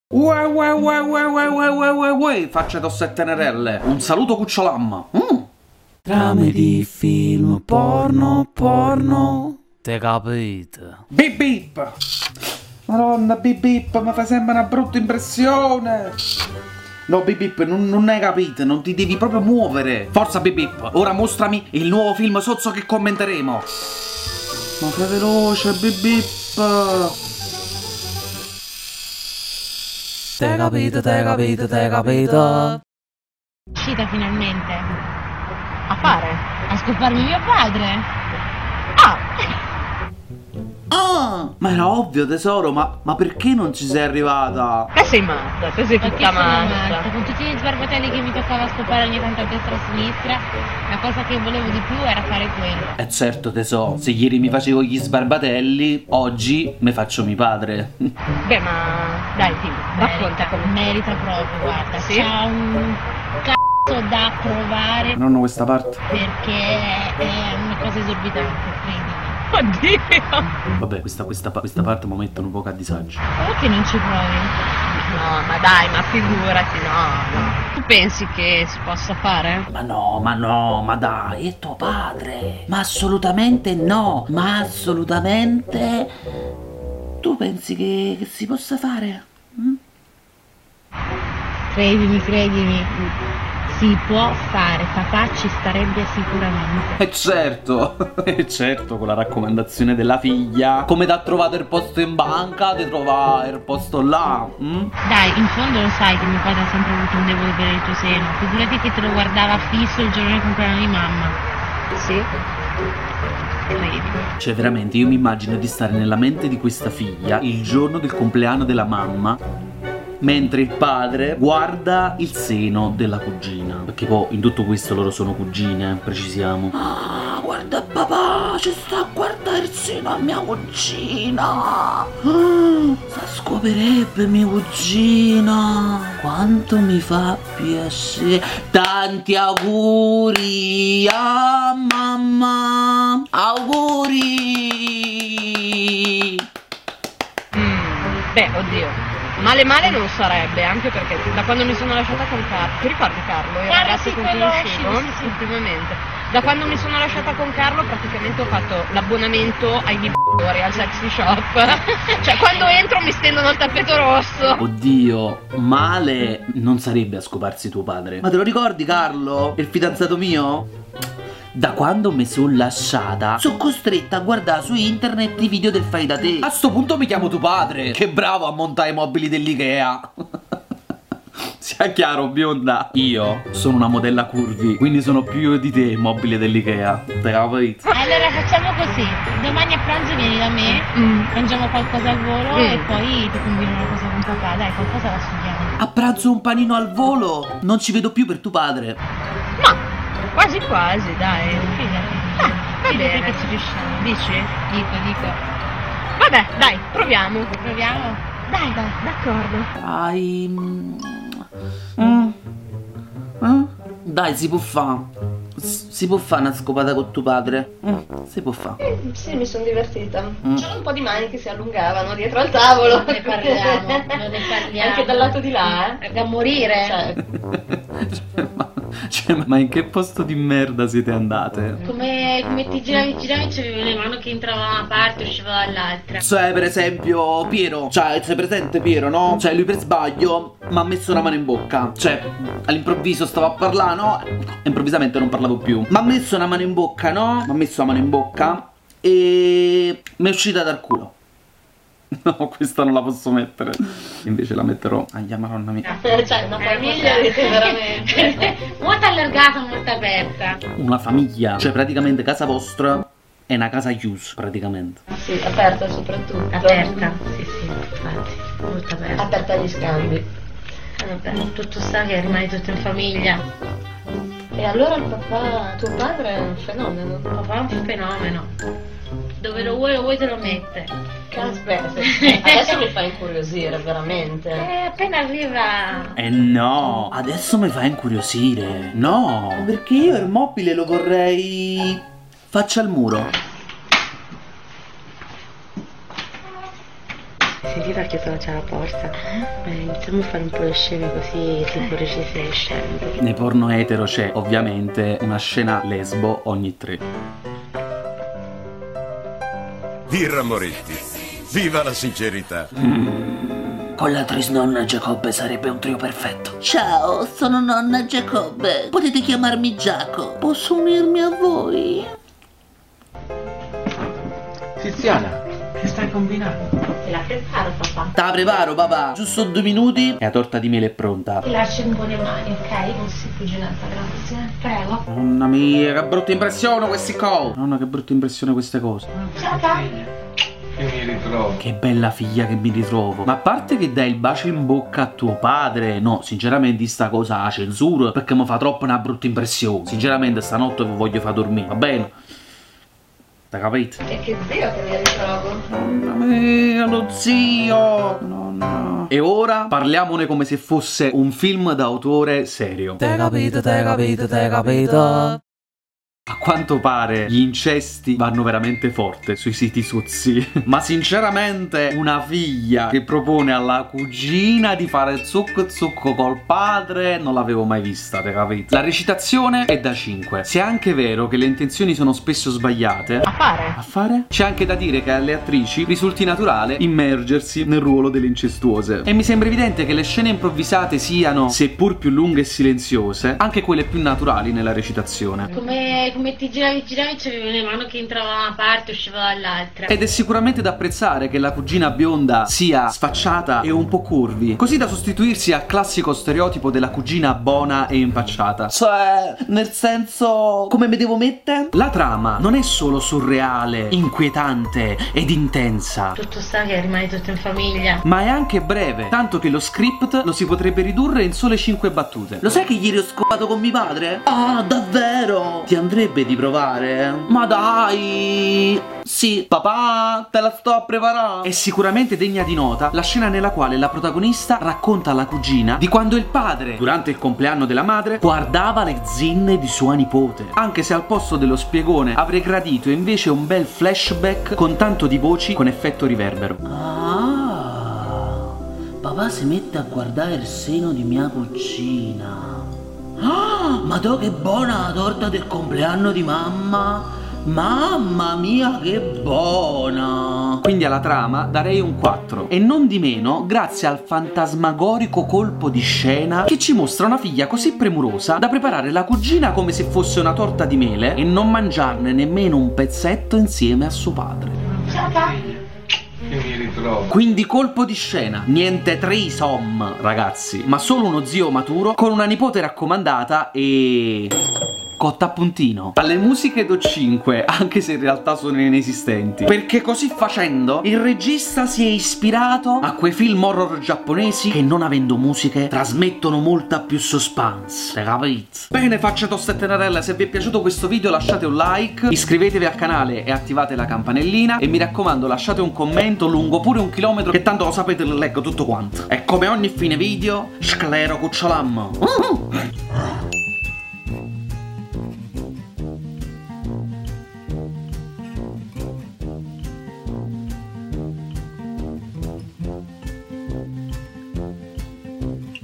TRAME DI FILM P0RNO #9 - Ragazza chiede alla cugina di farlo con suo padre
Ragazza chiede alla cugina di farlo con suo padre.

UEUEUE Facce toste e tenerell'!
Oggi vi porto la nona puntata della mia serie "trame di film p0rno" (per chi non la conoscesse è una serie ideata da me nella quale faccio recensioni di film p0rno)!
È una recensione di film p0rno trash fatta in modo, spero, creativo e cucciolammo come piace a voi ♥
Oggi si parlerà del film di una ragazza che chiede alla cugina di farlo con suo padre (suo zio)
Vabbè raga' non si può capire xD

Un abbraccio cucciolammo a tutti!
Il vostro John ♥